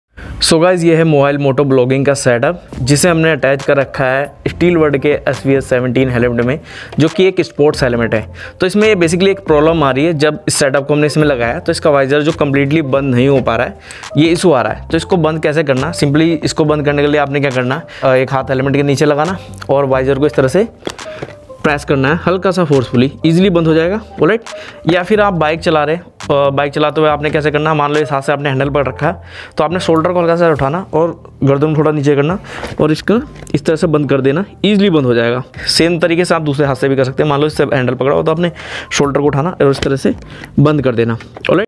Steelbird SBH 17 Helmet Visor Issue with Motovlog Setup
Mobile Motovlogging Set-up

○ Mobile Motovlogging Set-up

Mic
○ Boya BYM1 Mic

Helmet
○ Steelbird SBH-17 Terminator Helmet

Queries Solved:-
1) How To Motovlog With Mobile
2) Steelbird SBH 17 Helmet Visor Not Closing with motovlogging setup
3) Motovlogging Set-up ke Steelbird SBH 17 ka visor band nhi ho rha

○ Microphone:
○ Microphone Boya M1 Pro
○ Microphone under 2000
○ Go Pro For Moto Vlogging

Must watch